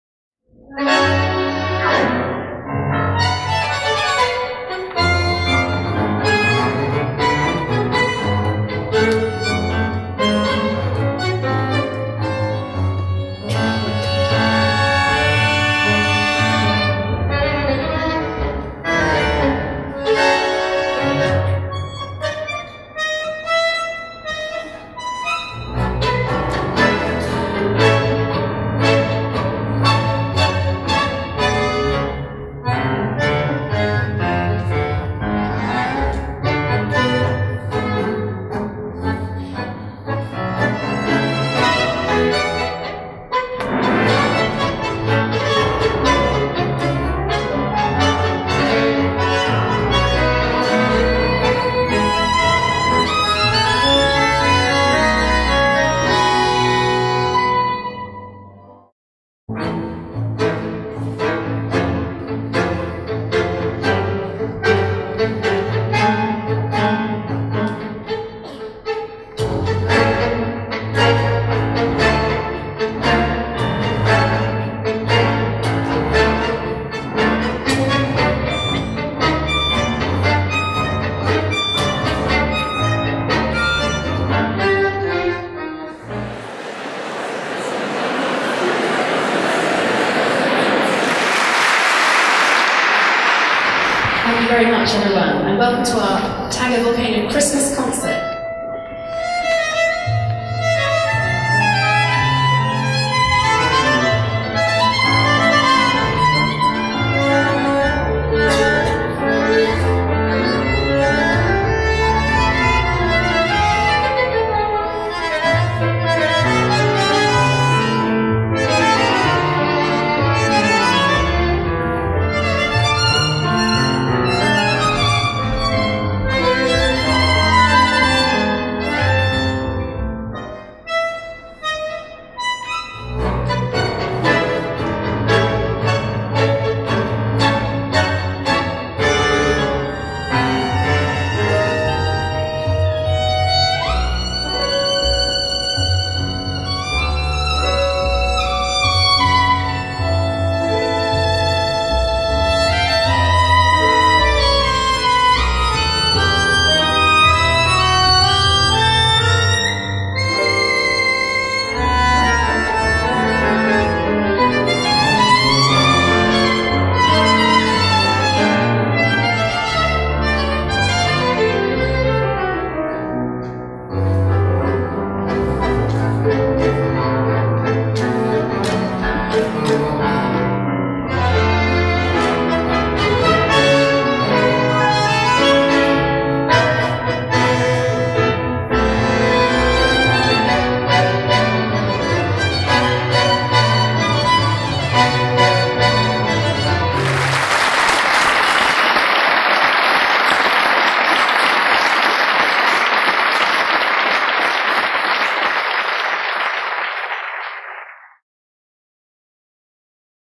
Tango Volcano - Performance - The Bridgewater Hall
Watch Tango Volcano rehearse and perform at The Bridgewater Hall, Manchester.